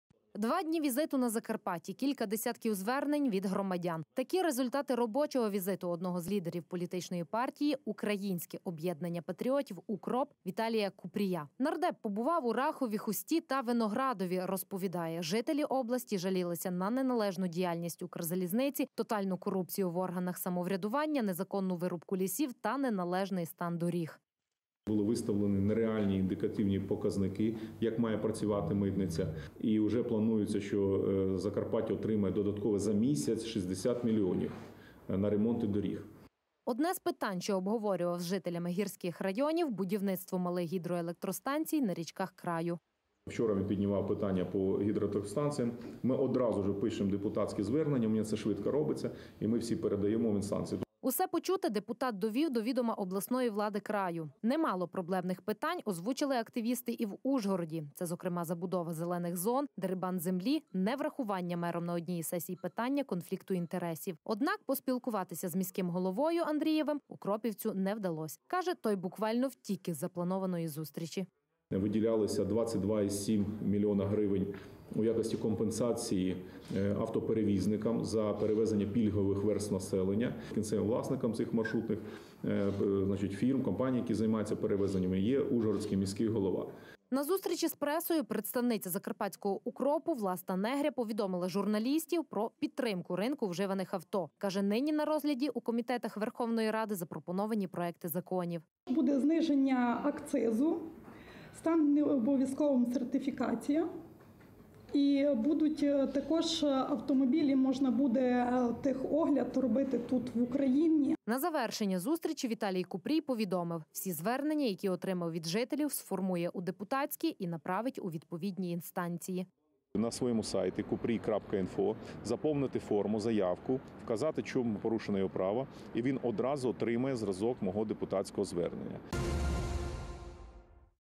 З візитом на Закарпатті народний депутат Віталій Купрій
З візитом на Закарпатті перебував народний депутат Віталій Купрій. Два дні візиту Закарпаттям кілька десятків звернень від громадян, такі результати робочого візиту одного з лідерів політичної партії «Українське об’єднання патріотів – УКРОП», Віталія Купрія. Нардеп побував у Рахові,Хусті та Виноградові, розповідає жителі області скаржилися на неналежну діяльність «Укрзалізниці», тотальну корупцію в органах самоврядування, незаконну вирубку лісів та неналежний стан доріг. Одне з питань, що обговорював з жителями гірських районів – будівництво малих гідроелектростанцій на річках краю. Усе почуте депутат довів до відома обласної влади краю. Не мало проблемних питань озвучили активісти і в Ужгороді: це зокрема забудова зелених зон, дерибан землі, не врахування мером на одній із сесій питання конфлікту інтересів… Однак поспілкуватися з міським головою Андріївим укропівцю не вдалось, каже той буквально, втік із запланованої зустрічі. На зустрічі з пресою, представниця Закарпатського Укропу, Власта Негря, повідомила журналістам, про підтримання ринку вживаних авто. Каже нині на розгляді у комітетах ВР запропоновані проекти законів. На завершення зустрічі Віталій Купрій повідомив: всі звернення, які отримав від жителів, сформує у депутатські і направить у відповідні інстанції.